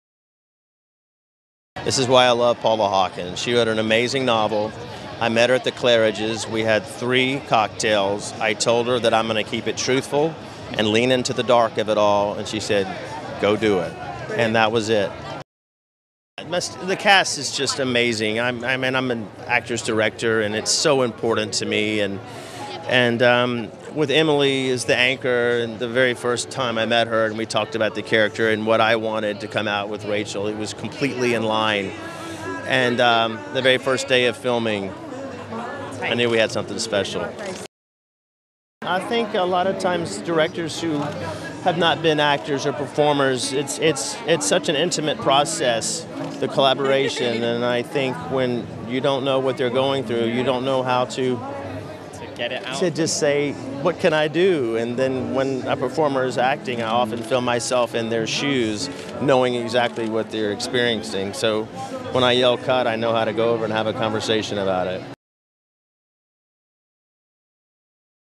Tate Taylor Interview The Girl On The Train Premiere
Red Carpet News TV talks to Emily Blunt, Luke Evans, Haley Bennett and director Tate Taylor at the world premiere of drama The Girl on The Train in London, based on the international bestselling book written by Paula Hawkins.